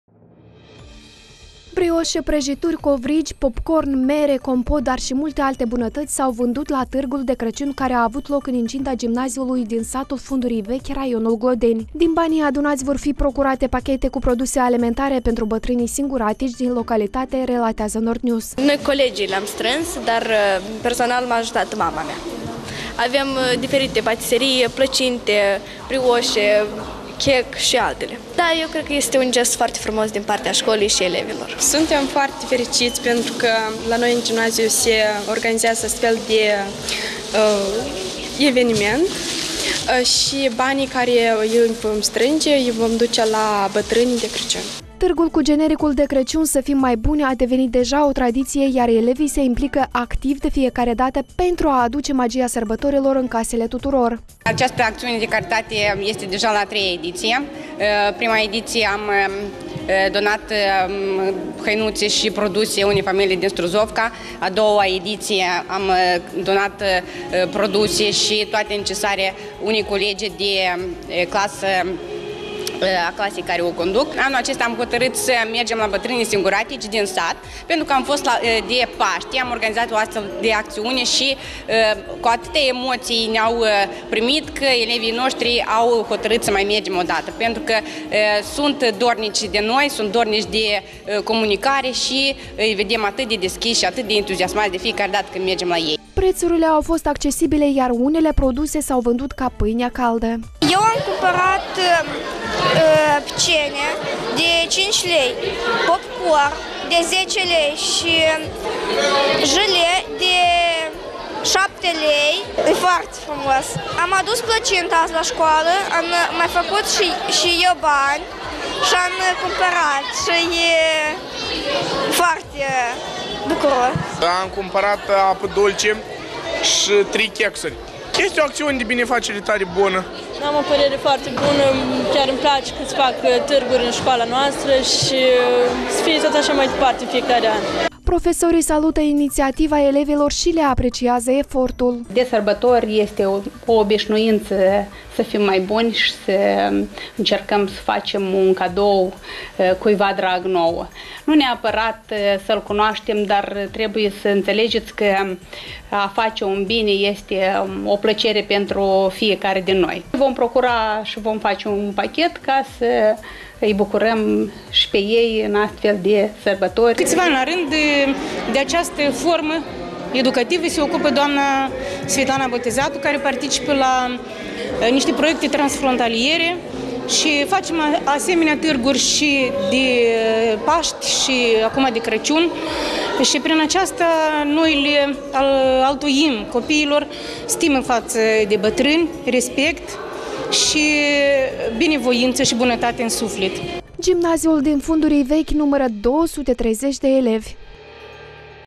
Târgul „De Crăciun să fim mai buni”! – o tradiție în satul Fundurii Vechi – AGRO TV News
Brioșe, prăjituri, covrigi, popcorn, mere, compot, dar și multe alte bunătăți s-au vândut la târgul de Crăciun, care a avut loc în incinta gimnaziului din satul Fundurii Vechi, raionul Glodeni. Din banii adunați vor fi procurate pachete cu produse alimentare pentru bătrânii singuratici din localitate, relatează NordNews.